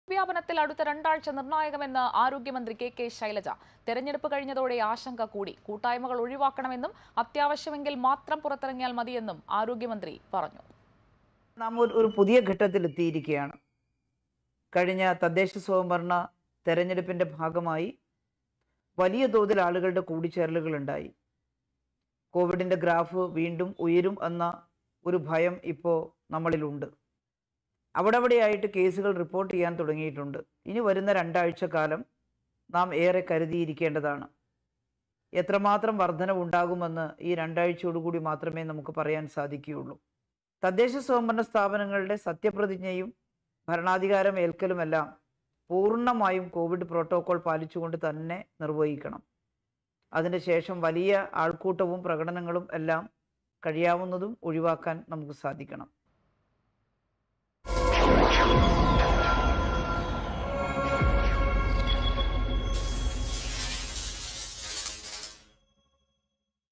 'കൊവിഡ് വ്യാപനത്തില്‍ അടുത്ത രണ്ടാഴ്ച നിര്‍ണായകം' | Covid 19
തെരഞ്ഞെടുപ്പ് കഴിഞ്ഞതോടെ ആശങ്ക. കൂട്ടായ്മകള്‍ ഒഴിവാക്കണം, അത്യാവശ്യമെങ്കില്‍ മാത്രം പുറത്തിറങ്ങുക'; കൊവിഡ് വ്യാപനത്തില്‍ മുന്നറിയിപ്പുമായി ആരോഗ്യമന്ത്രി